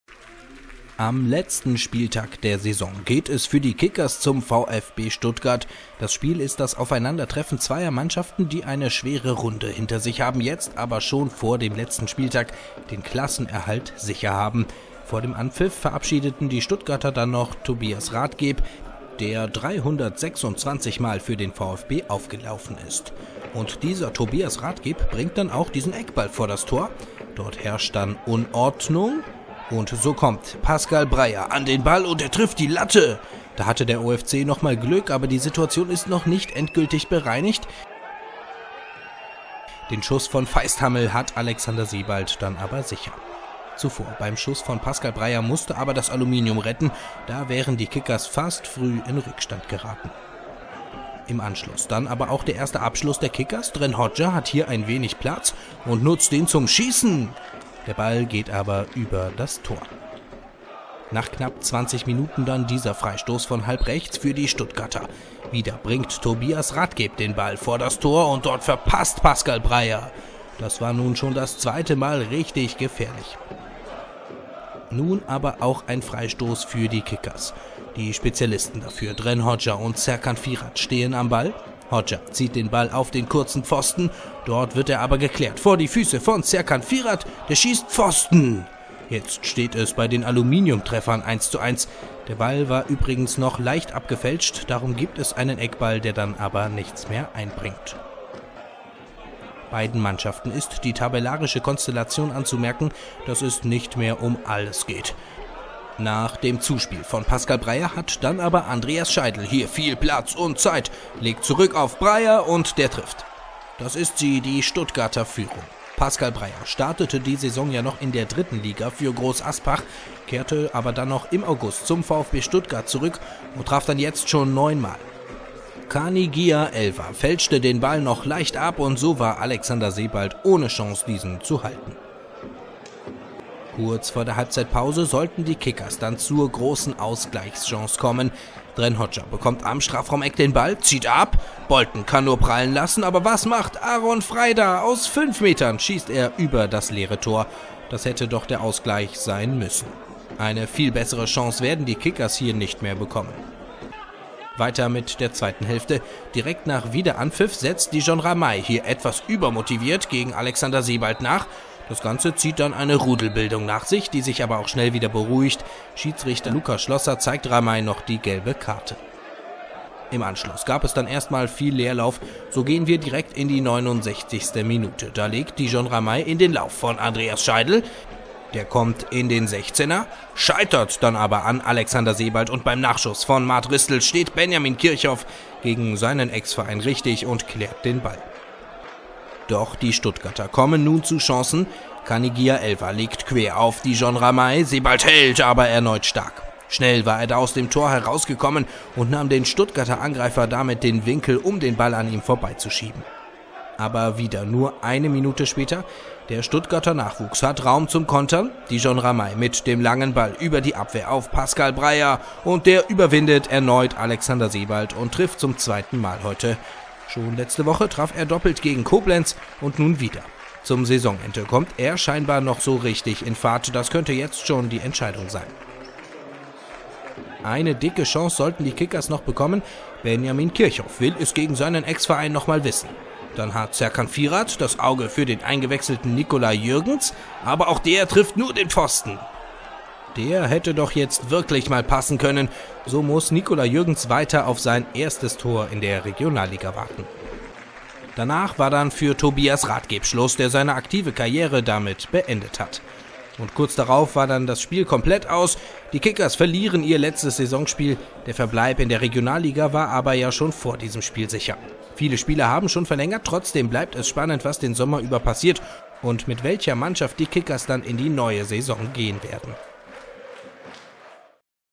VfB Stuttgart II gegen den OFC: Die Höhepunkte
- Stuttgart, 21. Mai 2017 - Im letzten Saisonspiel verlieren die Offenbacher Kickers gegen die zweite Mannschaft des VfB Stuttgart mit 0:2.